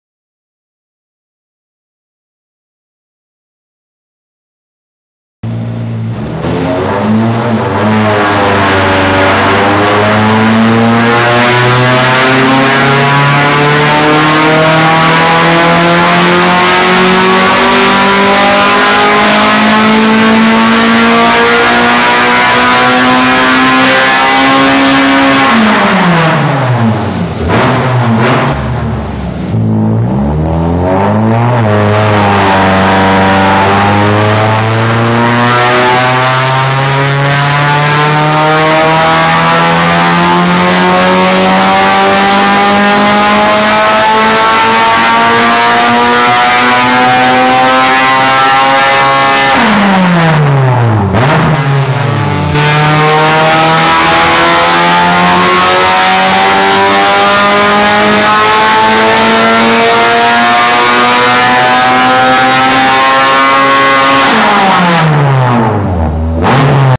RaceEKApril2k9
April 2009
DMT Civic Race Car
Preparing for 2009 Racing season
AEM EMS

Tuned by Dynamotorsports
Visit Dynamotorsports.ca for more details